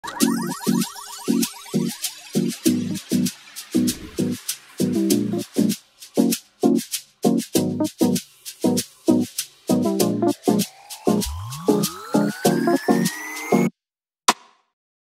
Caroline with our Aqua Resist | MAKE UP FOR EVER
Our AquaResist Smoky Shadow is a 3-in-1 shadow, kohl and liner stick that delivers intense pigment and cresaless, waterproof, smudge-proof wear.

Recreate Caroline's summer eye look using AquaResist Smoky Shadow in the shade Peony and AquaResist Color Ink in the shade Matte Wood.

Video by Natalie Blanco.